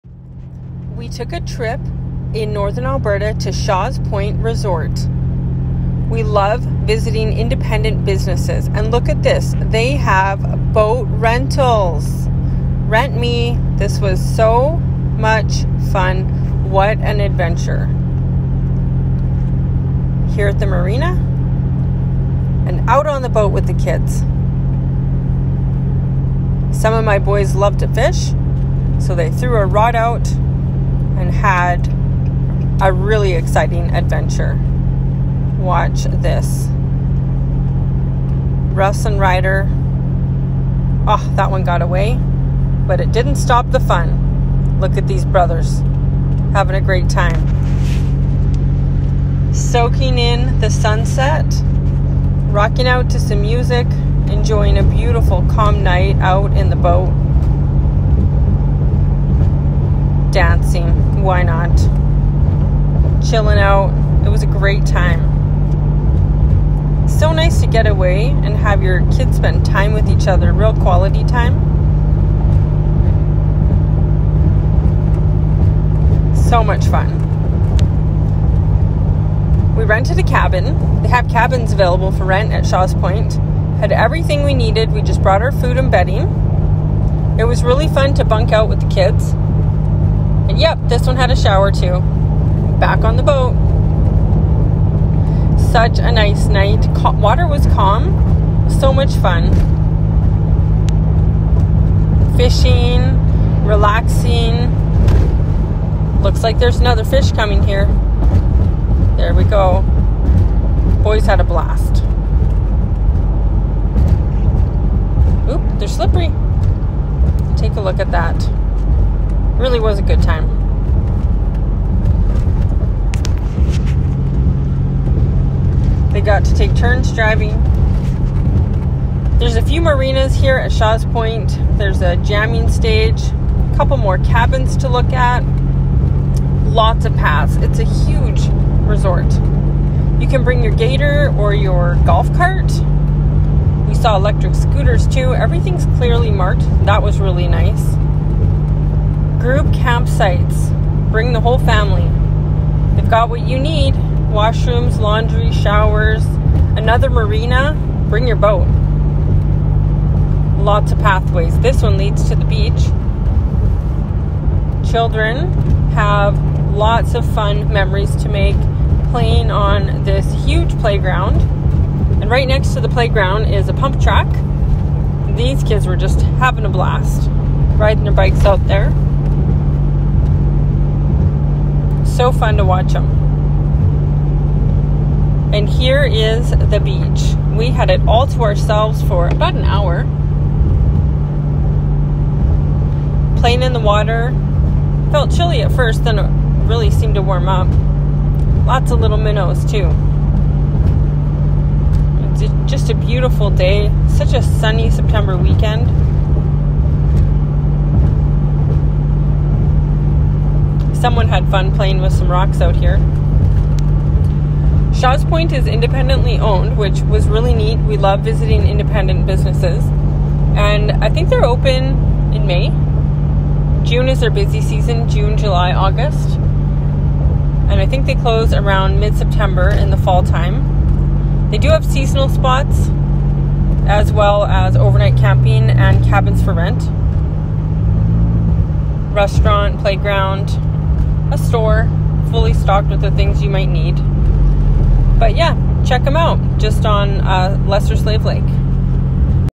Adventure’s at Shaw’s Point Resort
We had a fun family adventure in Northwrn Alberta at Shaw’s Point Resort, highly recommend!!
They have overnight camping, seasonal lots, Cabin rentals, BEACH, swimming, restaurant, fully stocked store, firewood, several marinas, boat rentals, playgrounds, a bike track. So much fun!